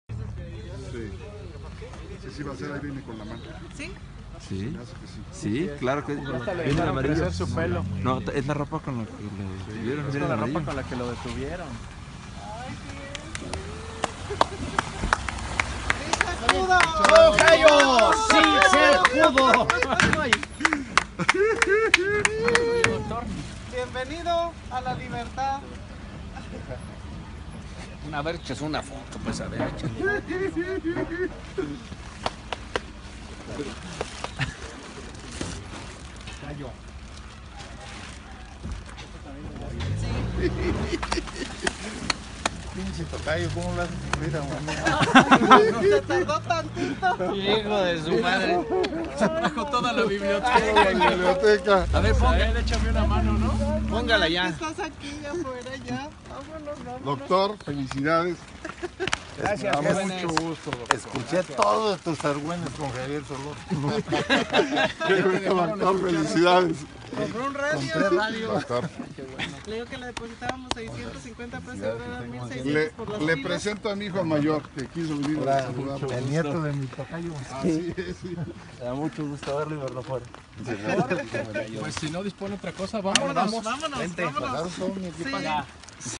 Mireles libre, IMÁGENES EXCLUSIVAS al salir de cárcel de Nayarit
Después de 3 años preso, el fundador de Autodefensas Dr. José Manuel Mireles sale en libertad.

Es recibido por Manuel Antúnez y Daniel Moncada de Movimiento Ciudadano, por su hermana y por su equipo legal encabezado por Ignacio Mendoza.